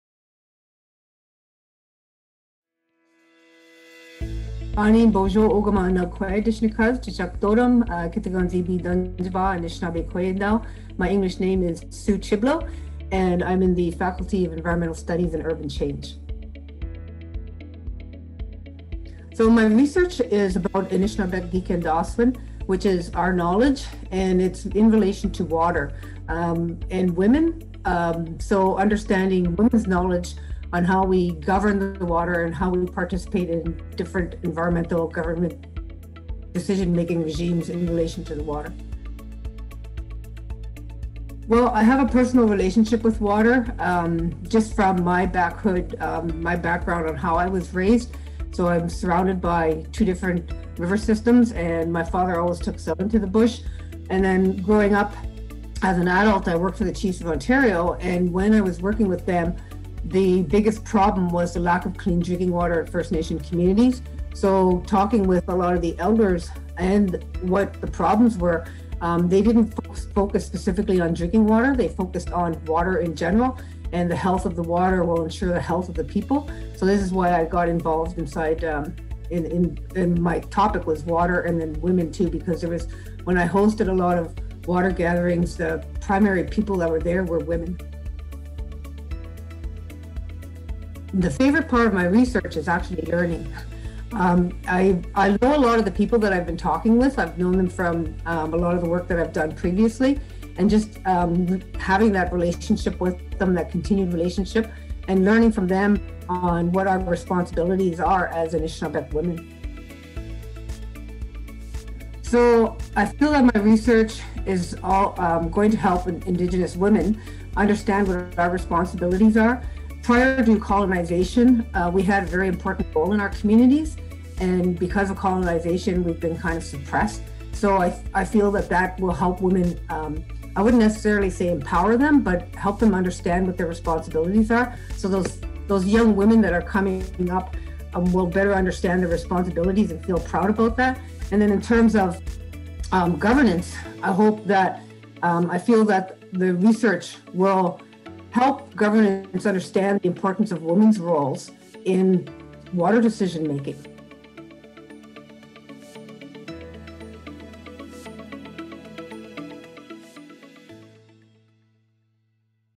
Knowledge Now - Grad Student: Susan Chiblow
Susan Chiblow (Doctoral student in Environmental Studies) talks about her research on “N’be Kendaaswin” (water knowledge). Her research highlights the vital role of water in the well-being of indigenous communities and the importance of Anishinabek and other Indigenous women and their knowledge in water governance and decision making.